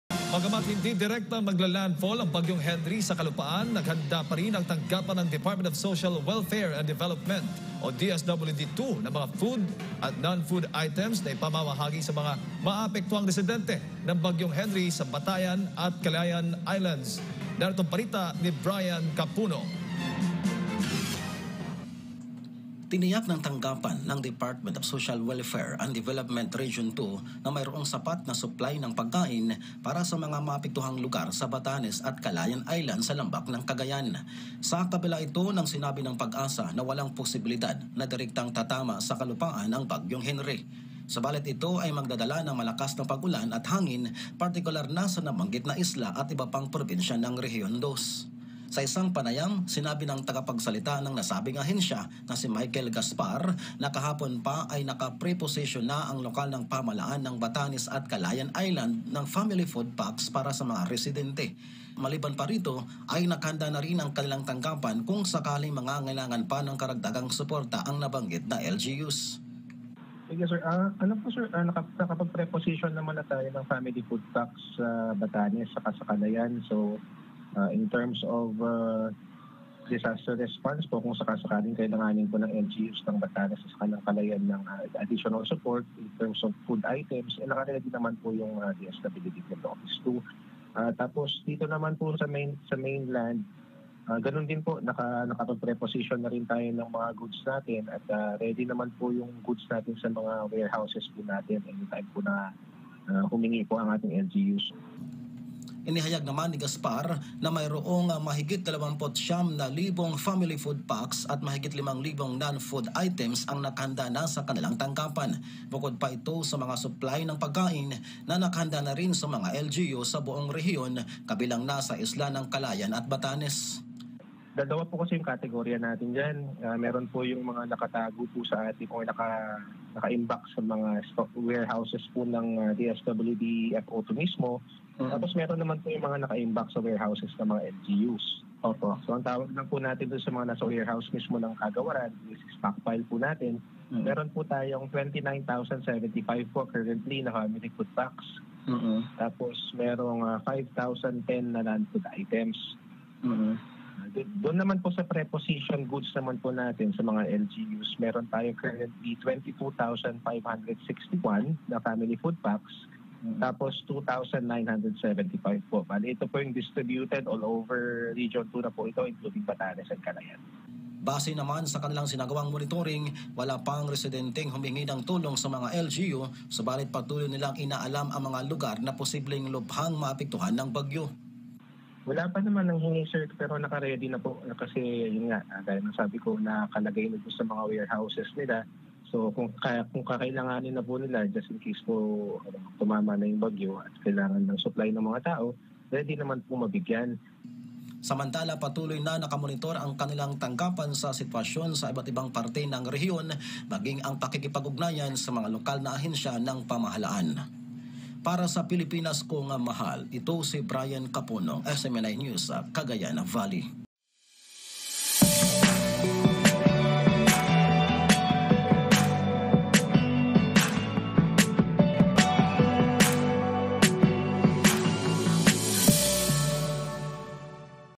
DSWD-2, tiniyak ang sapat na suplay ng pagkain sa mga maaapektuhan ng bagyong Henry
DSWD-2, tiniyak ang sapat na suplay ng pagkain sa mga maaapektuhan ng bagyong Henry sa Batanes at Calayan Island | via Brayan Capunong